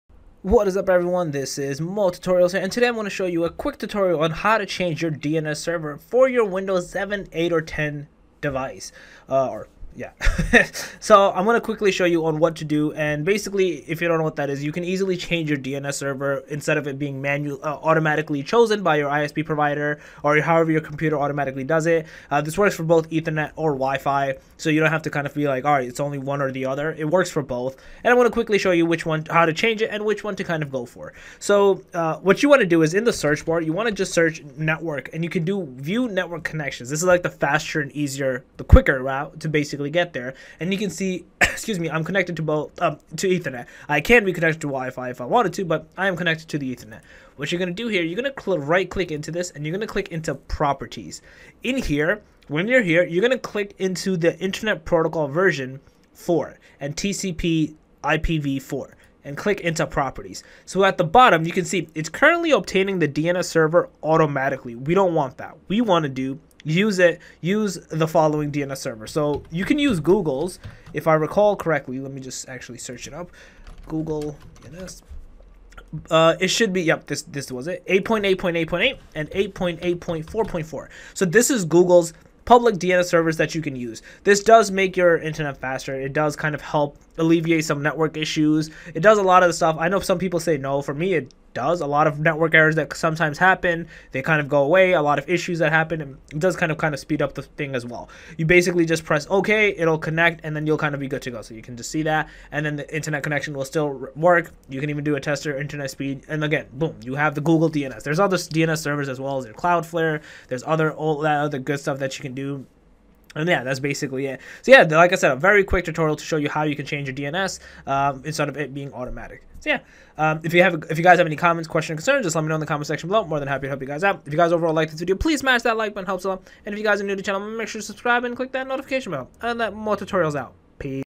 How To Change Windows 10 DNS Server For Faster Internet In 2021
In This Video, I Will Show You How To Change Windows 10 DNS Server For Faster Internet In 2021